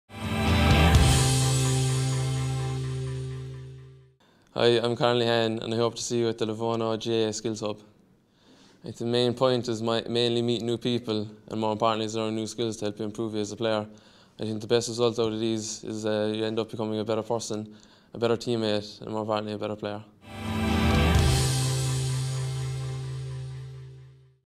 Cork's Conor Lehane on being a better teammate at Lenovo GAA Skills Hubs
The Lenovo GAA Skills Hub is a nationally co-ordinated Gaelic Games programme of skill development activities taking place in a positive playing environment, providing boys and girls aged 13 -15 with the opportunity to derive maximum enjoyment from their involvement in Gaelic Games.